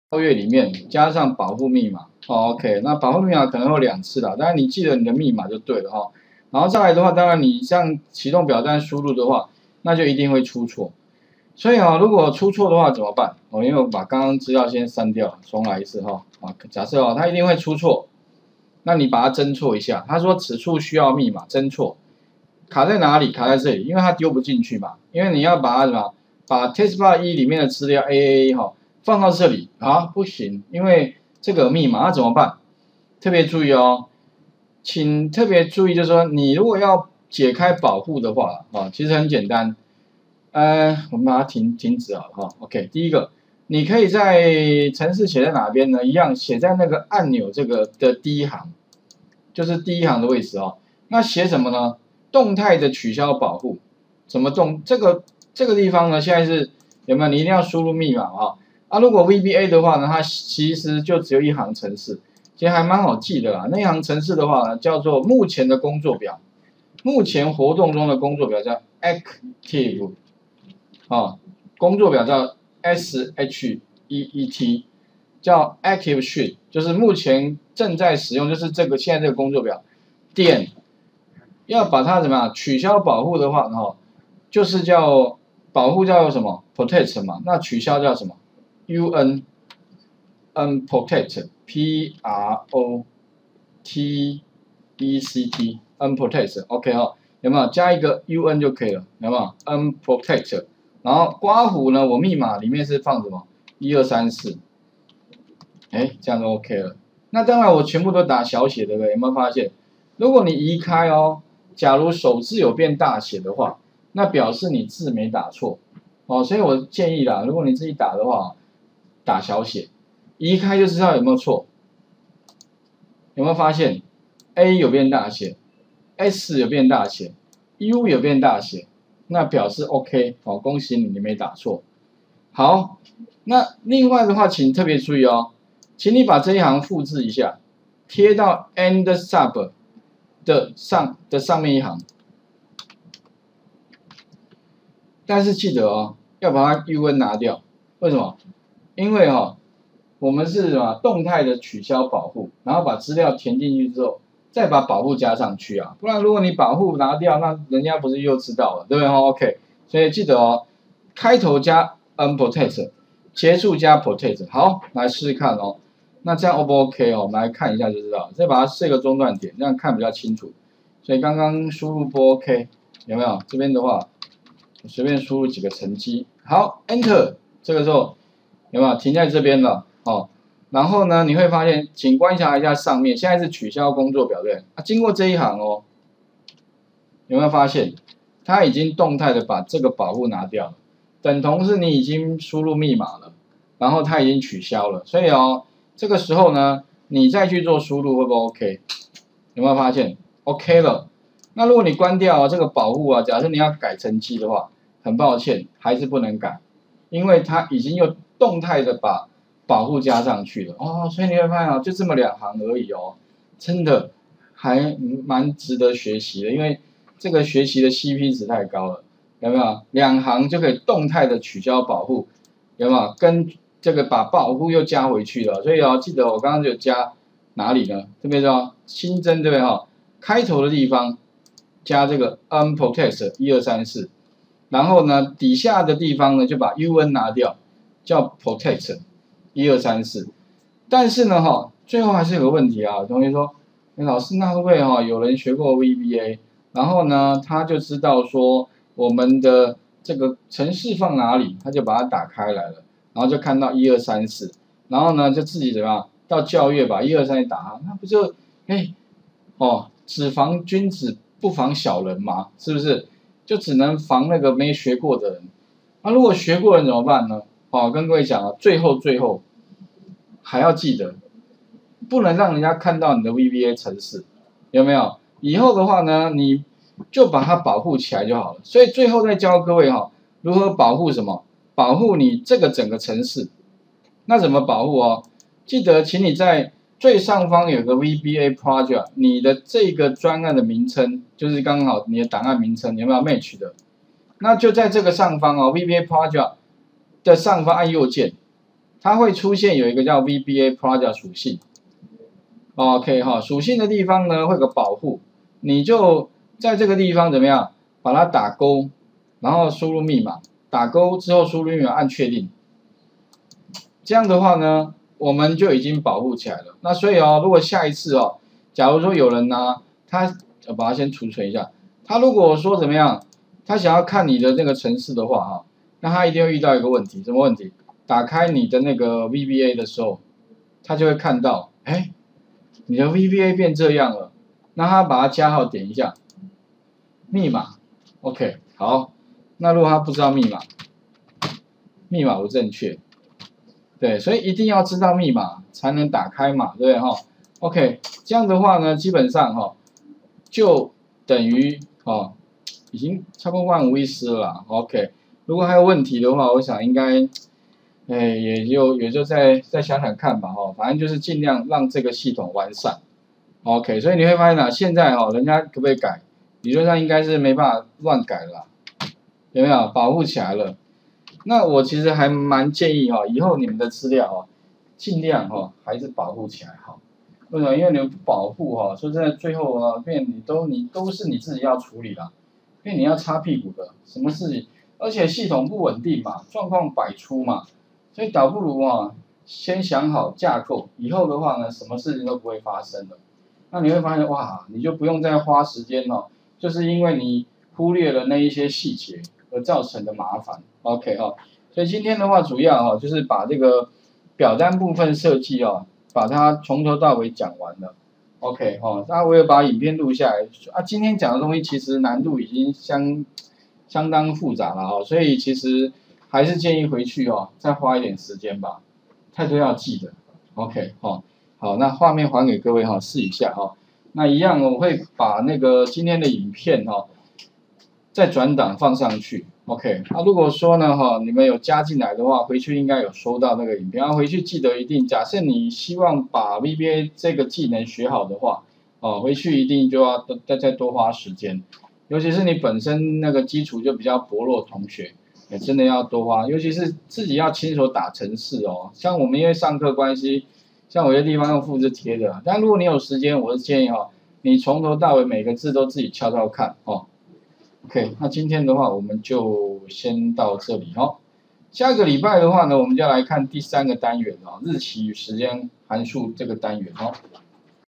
09 保護工作表與保護VBA專案
東吳EXCEL VBA與資料庫雲端設計112第4次(樞紐分析表統計住宅竊盜案件區與路&表單功能與設計&啟動表單&VBA重劃框線與防呆一防止未輸入資料&ENTER自動輸入&保護工作表與保護VBA專案)

上課內容：
01_重點回顧與住宅竊盜統計
02_用樞紐分析表統計住宅竊盜案件區與路
03_演示表單功能與設計
04_設計表單由外而內由左到右
05_啟動表單的兩種方式
06_新增按鈕的VBA程式說明
07_VBA重劃框線與防呆一防止未輸入資料
08_判斷數字與0到100之間與ENTER自動輸入
09_保護工作表與保護VBA專案

上課用書：
Excel VBA一點都不難：一鍵搞定所有報表
作者： Excel Home
出版社：博碩
出版日期：2013/06/26
定價：380元

吳老師  109/10/26